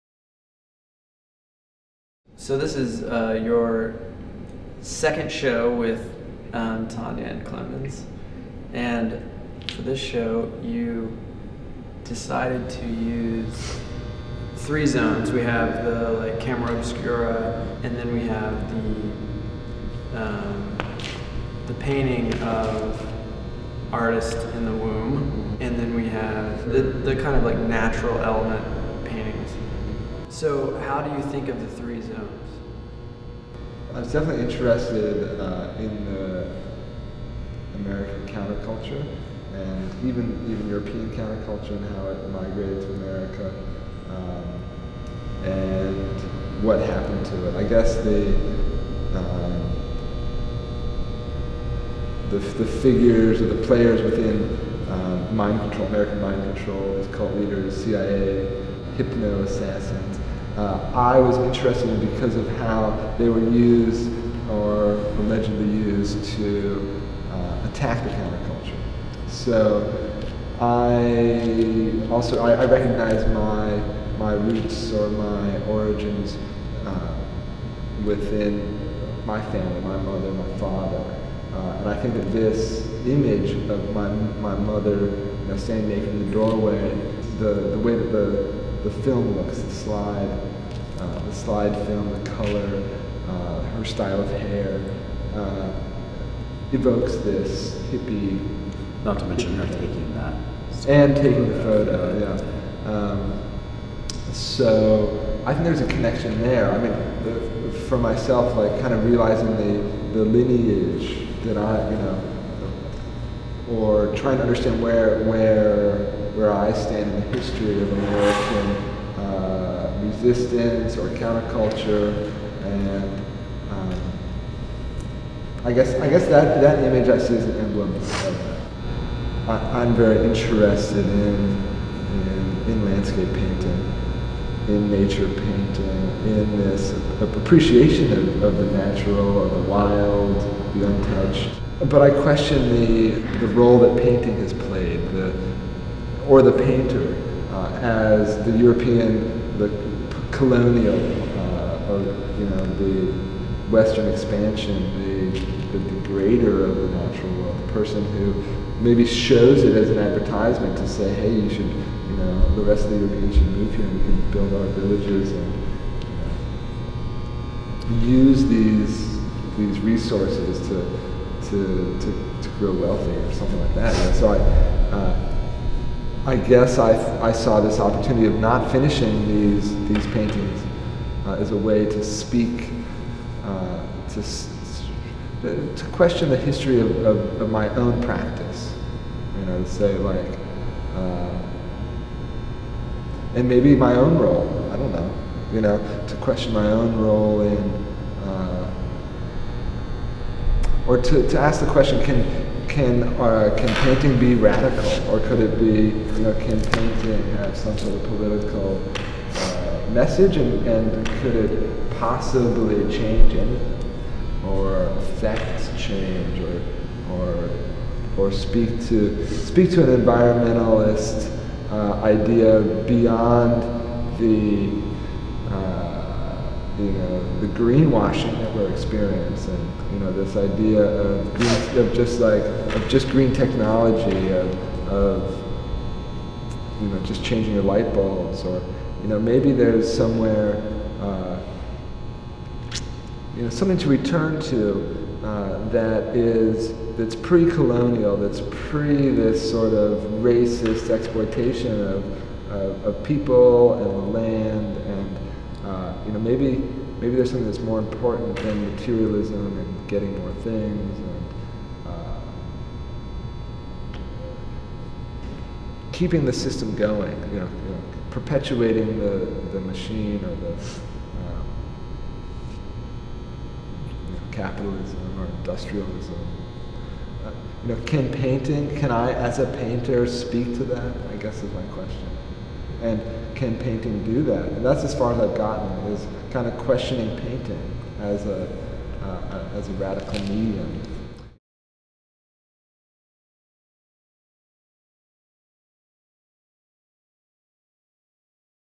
Paul Jacobsen Interview by Grayson Cox and Jesse Weiss
Grayson Cox and Jesse Weiss Interview Paul Jacobsen for "Mouthpiece" at Klemens Gasser & Tanja Grunert, Inc.